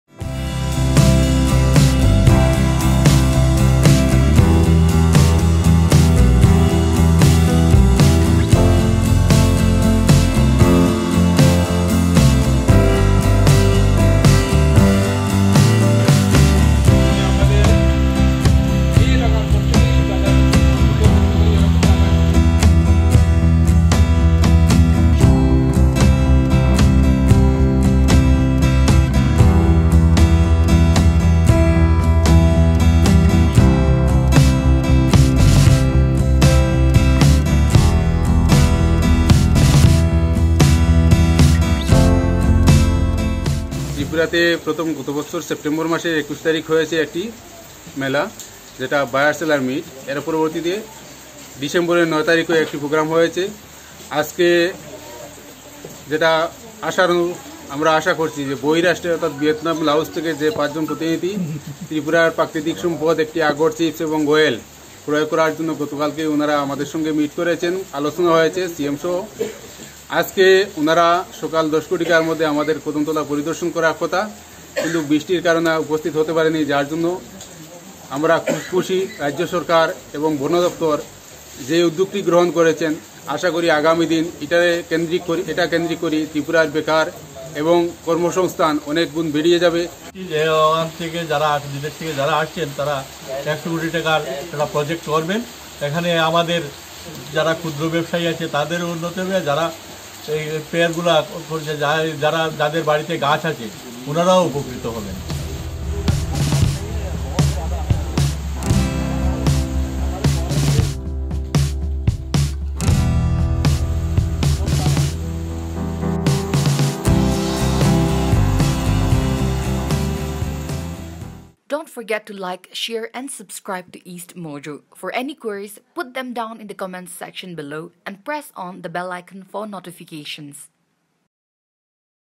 Laos industrialists to invest in Agar industry in Tripura: CM Biplab Deb
Tripura Chief Minister Biplab Kumar Deb on Monday said industrialists from Laos have arrived in the state and gave a detailed presentation of the prospects of the Agar industry. Addressing the central function of Kabi Pranam to mark the 161s birth anniversary of Rabindranath Tagore, Deb said, “Just hours back, I have attended a meeting of industrialists who have come over here from Laos to study the prospects of Agar based industries in the state. The company has an annual turnover of Rs 5,000 crore, and they are eager to make private investment in our state.”

Deb also informed the gathering that a team of officials led by the Chief Secretary recently visited countries like Vietnam and Laos to study the industrial aspects of Agarwood.

“We have 70 lakh trees all over the state. These plantations had been raised by the local communities without any support from the state government. For decades, the local communities were forced to sell the products derived from Agar plantations in black markets. They did not have any authorization,” said Deb, adding that his government formalized Agar trade in Tripura, which is now set to give the state’s economy a positive spin. “The team from Laos has given a commitment of private investment and assured us that all the Agar-based products would be produced here in Tripura,” the Chief Minister added.

Slamming the Left, Deb said, “The name of Tripura’s capital Agartala came from Agar. Everyone is aware of that. But the previous government never tried to acknowledge the sector. I have talked to Prime Minister Narendra Modi about the export quota. Today, 25,000 metric ton export quota is granted for us which never existed before.”

Deb said his government is tirelessly working to make Tripura the number one state. “Tripura is a small state. Several states have districts equal to the geographical area that my state holds. People may brand this as a weakness, but I see it as a strength. The web of administration is close to the people,” said Deb.